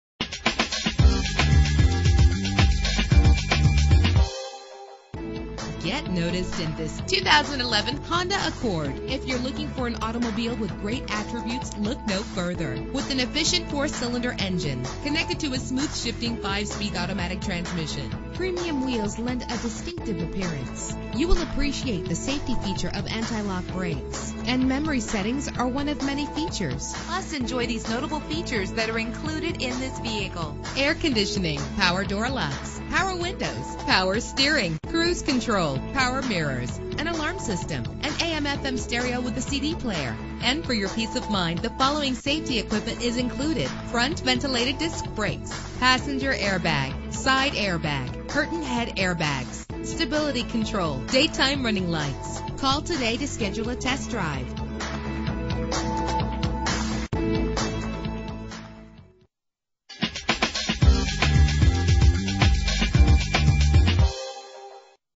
2011 Honda Accord - Old Bridge NJ
Year: 2011
Make: Honda
Model: Accord
Trim: 2.4 LX-S
Engine: 2.4 liter inline 4 cylinder 16 valve
Transmission: 5-Speed Automatic
Color: Alabaster Silver
Mileage: 10
Address:
1101 US highway 9